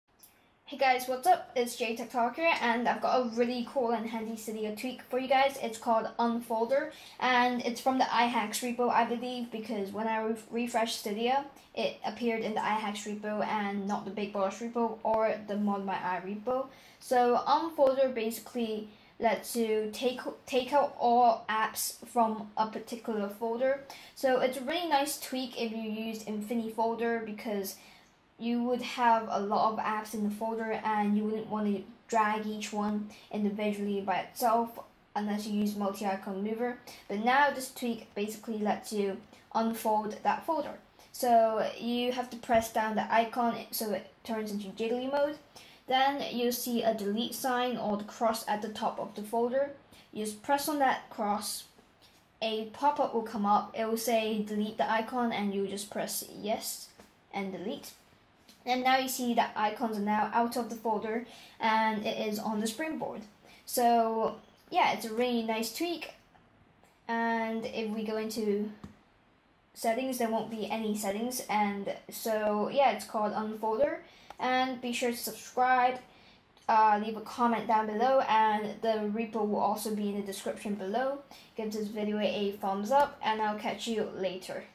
UnFolder Cydia Tweak - Unfold your iOS Folders in one move!
UnFolder is a really super handy tweak for those who like making folders and grouping apps. But when you start ungrouping them and want them on your springboard, UnFolder will help you do this in one move, you can now take all your apps out from a folder at once without needing to move them out individually. Simply, UnFolder deletes folders from the springboard